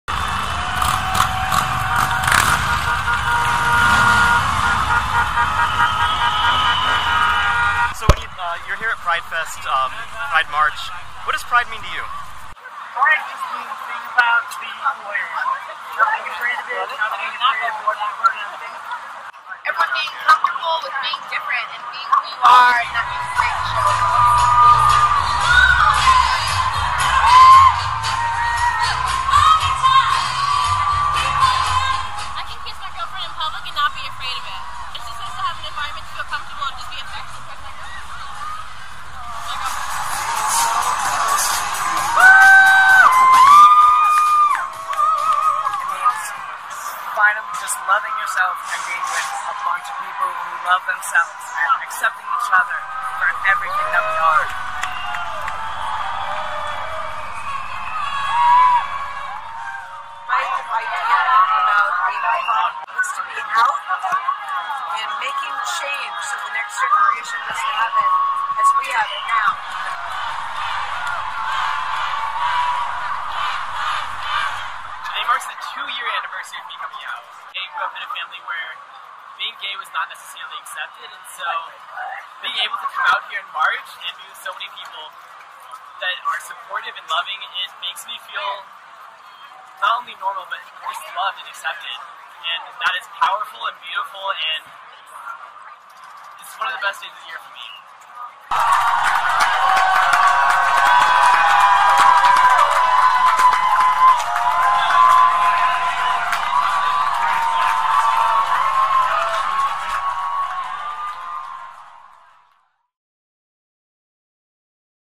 nycpride 2017 - 3D 360º
A short series of interviews intercut with footage from the 2017 New York City Pride March. Second experiment with 3D 360, exploring camera placement and interview style.

If you have a Google Cardboard or equivalent VR headset, view this with the YouTube app; turn on high quality and headset mode. If you have red/blue 3D glasses, view this in Anaglyph mode and drag the video to look around. The red lens should go on the left eye and the blue lens should go on the right.